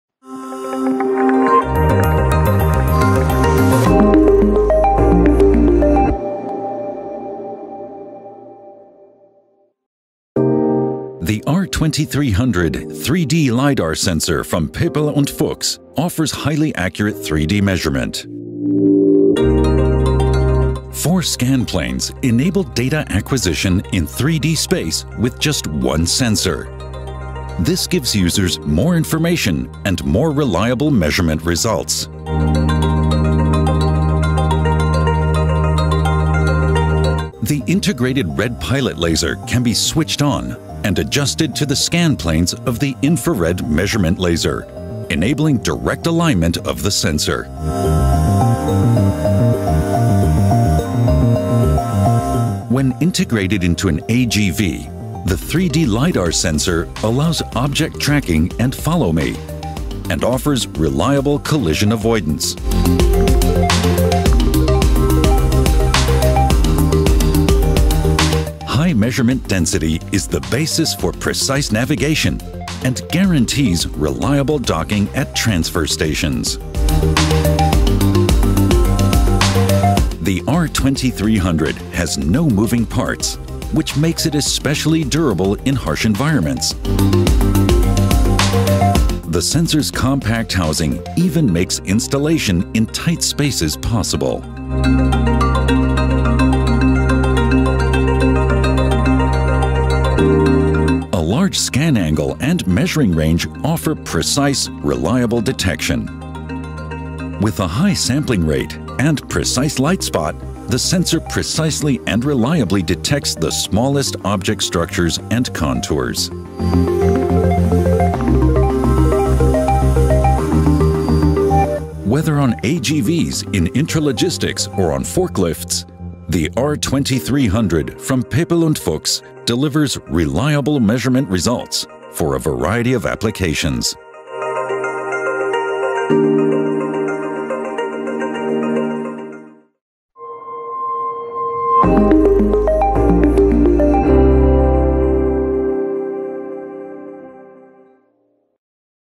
Escáner multicapa R2300 | Sensor LiDAR para la detección de objetos en 3D
El escáner multicapa R2300 ofrece cuatro planos de escaneo en un solo sensor LiDAR, aumentando así la fiabilidad de las mediciones en 3D. La animación muestra los cuatro planos de escaneo del sensor y la instalación simplificada usando el láser piloto conmutable y visible. Dos aplicaciones muestran al R2300 evitando colisiones en un sistema de transporte sin conductor y en la supervisión del área trasera.

¿Requieres más información?